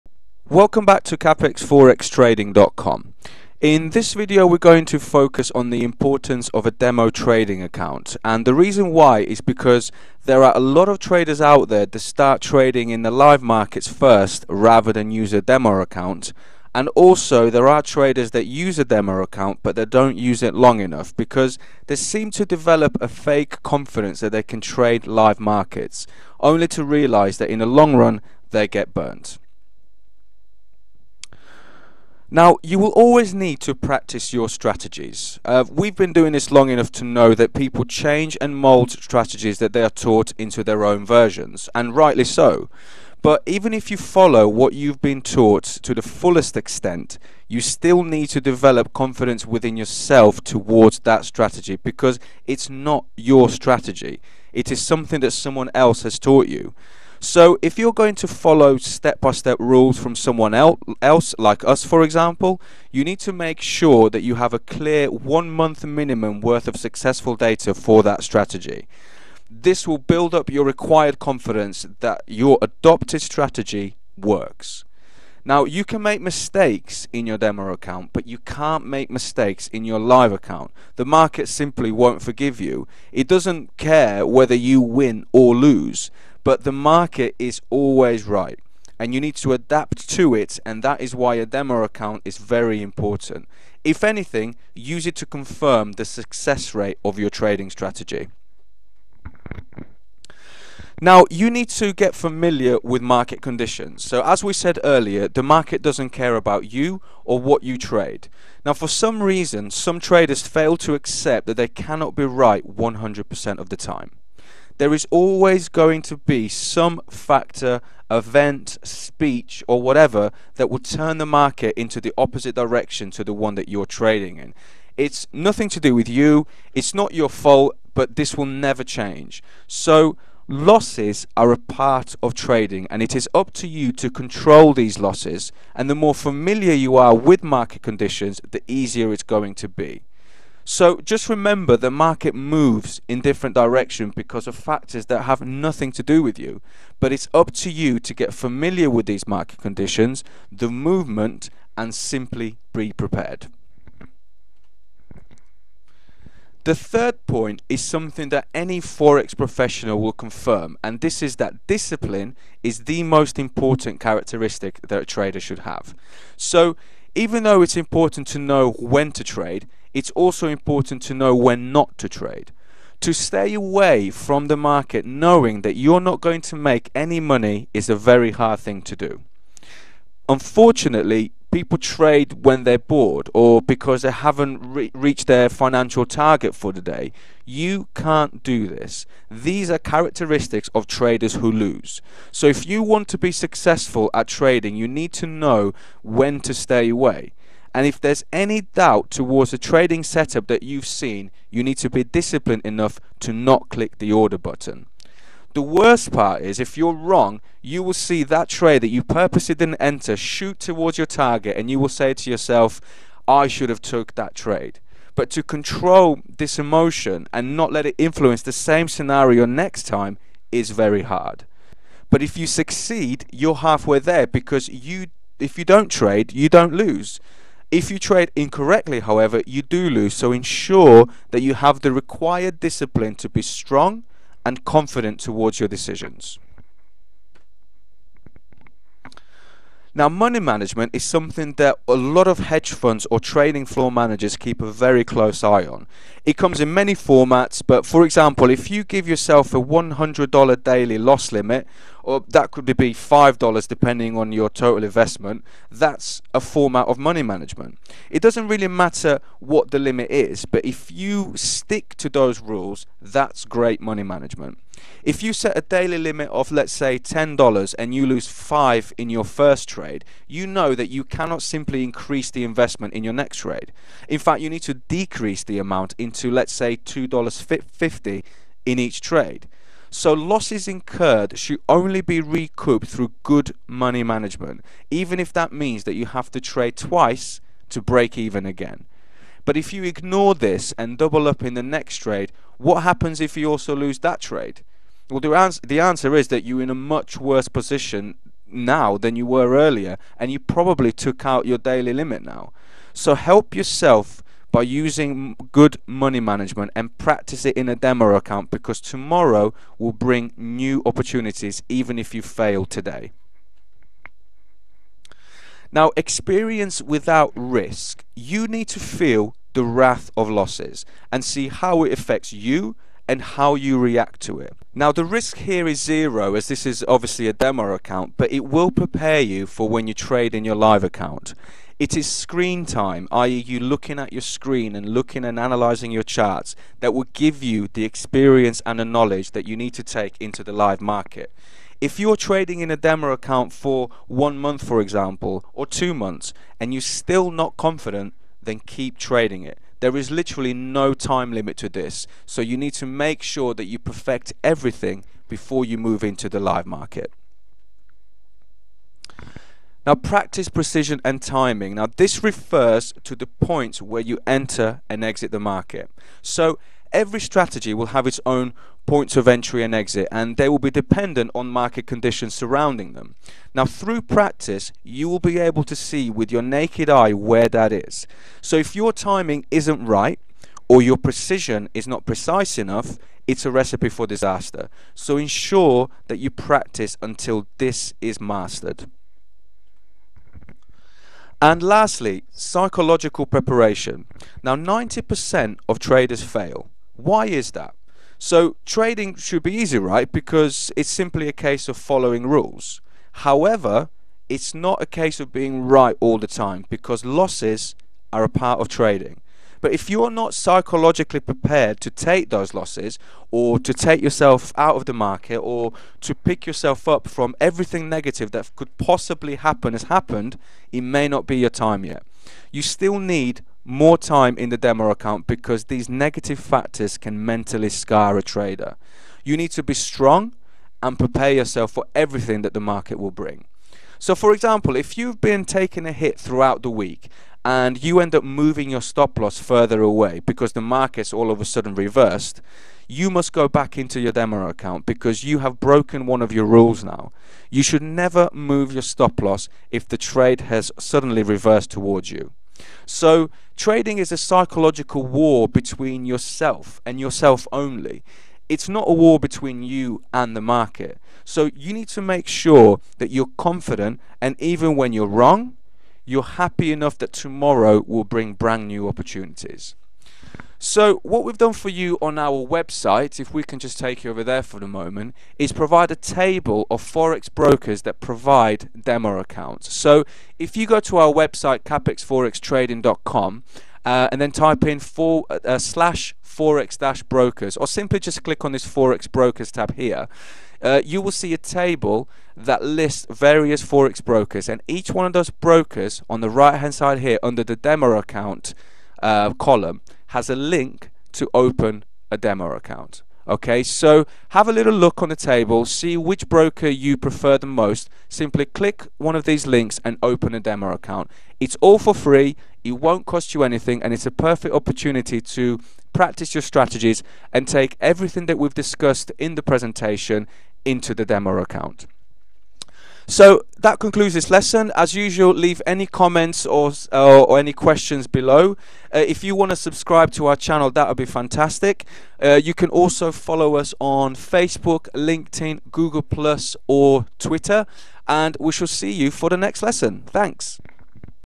6.5 Why you need a broker demo account and where to find one
Demo accounts are an absolute must for all traders - whether you are a beginner or a professional. The main purpose of having a demo trading account with a broker is to practice everything you are going to do in live markets, in a safe and risk free environment. This includes trading strategies, money management, appetite to risk and many other important factors.

The point here is to use the demo account to become a master of all aspects of your trading first, and then take that into live markets with full confidence. For example, you may have learnt about one of our strategies that we teach. Even if you followed all the rules step by step, it is not safe to trade it in live markets straight away. Instead, you need to practice all the aspects concerned with the strategy in your DEMO account first. From here, you will mould and re-design the strategy to suit your trading personality and take it into live markets only after a long period of successful trades.

In our video tutorial we explain in detail all the major aspects that you need to know about when it comes to demo accounts. Please ensure you watch this video before you sign-up to a live account because many traders tend to bypass this and get burnt in the live markets in no time at all. We also provide instructions on where to find brokers that provide demo accounts for free.